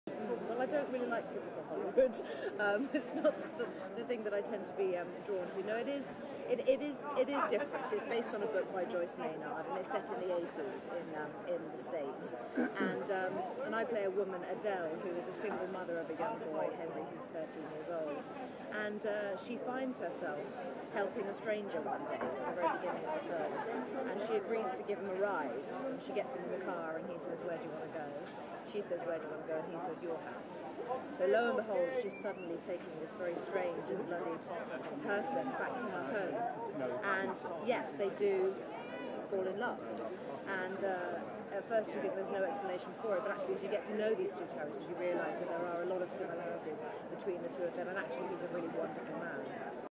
Kate Winslet
Quick interview with Kate Winslet on her recent film, Labor Day.
Credit: Words courtesy Sky News/Getty Images